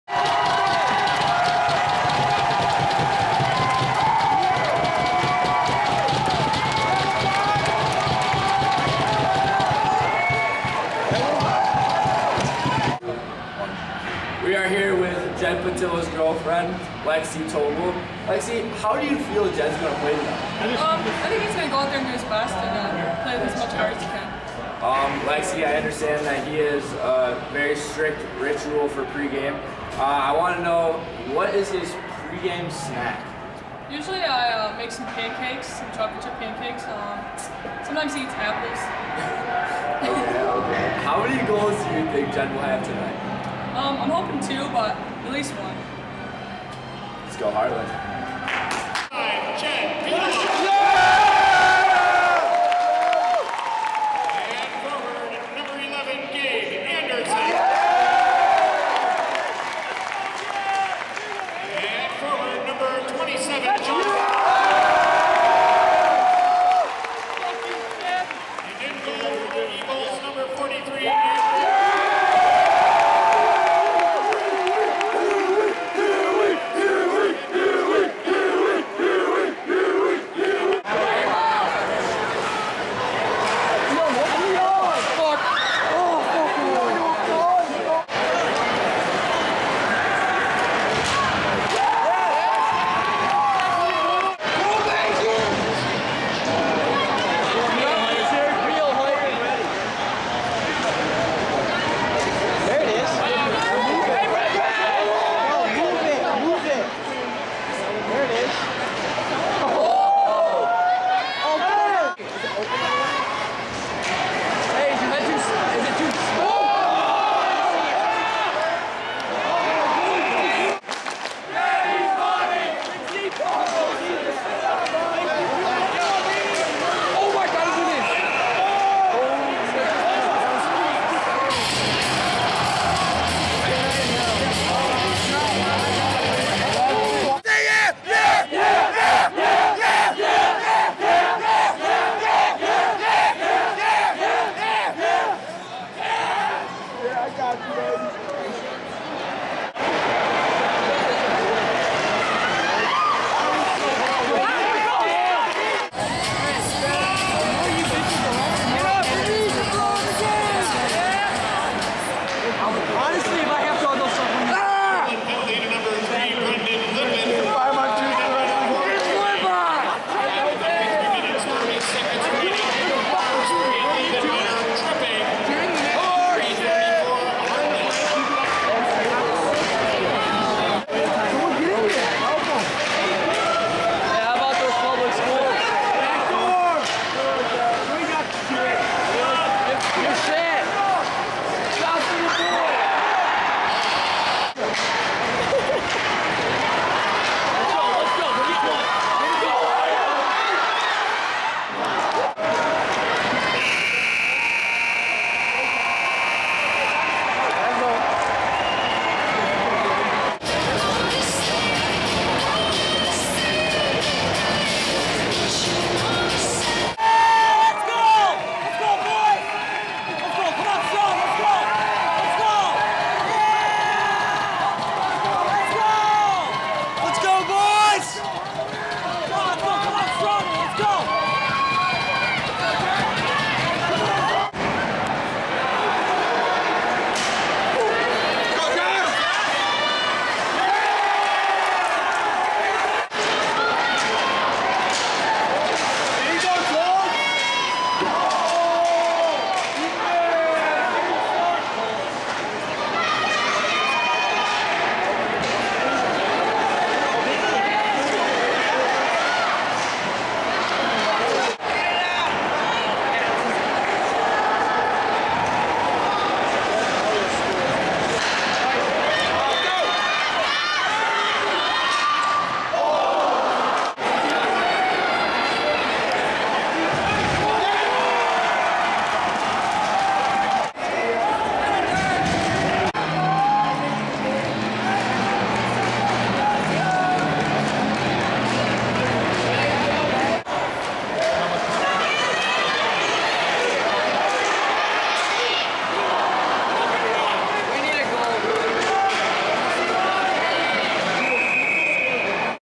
Hartland Varsity Hockey vs. Brother Rice
Dylan Couture, Tyler Thompson, Grant Prinzing, G Potter, Jake Eicher

Highlights from the MHSAA State SemiFinals